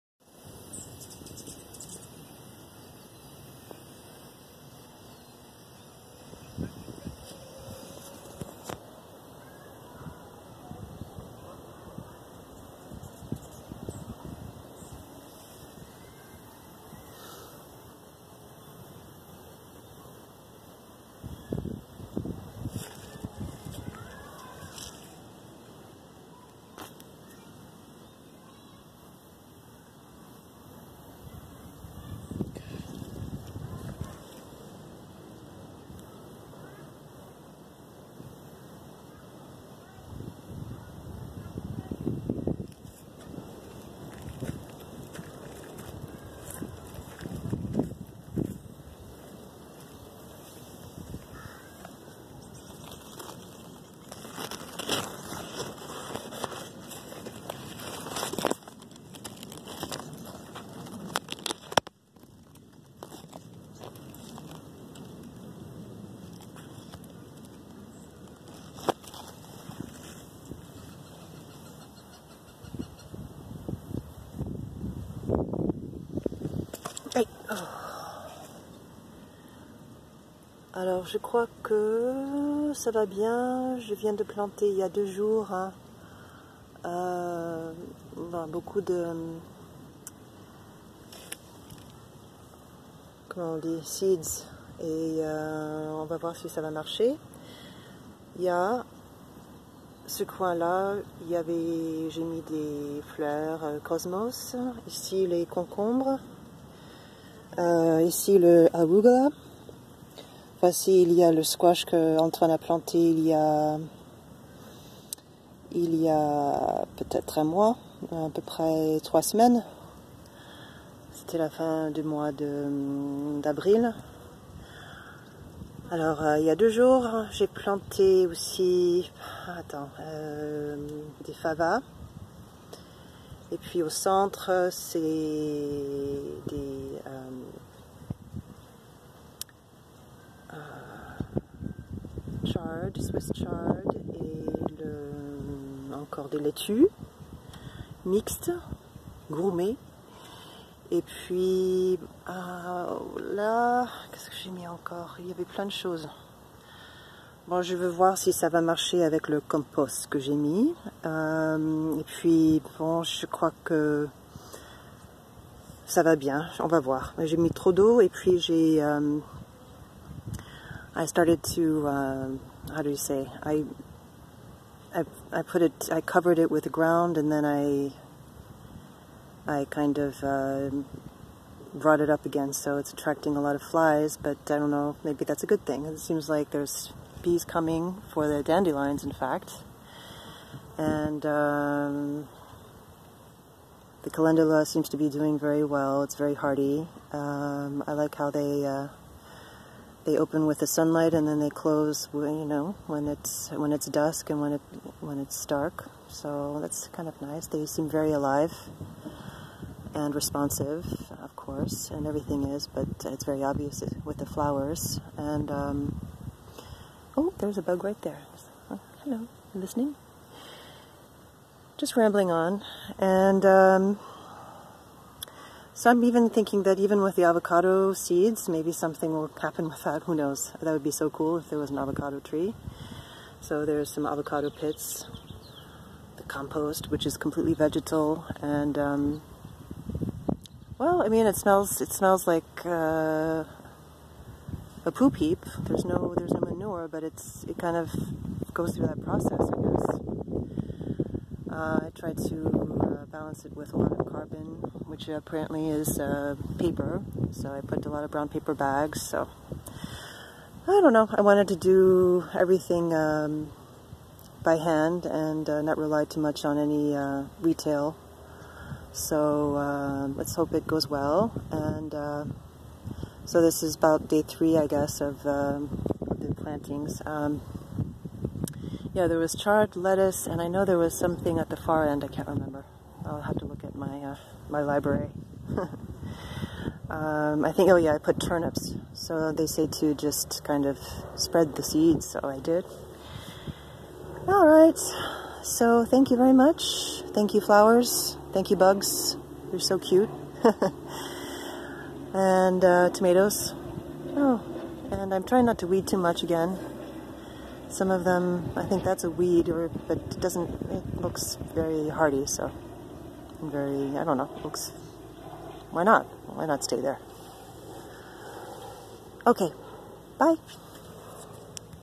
After planting seeds mid June 2019
Added compost to garden, planted various seeds, loving the green, 🌞 and bugs.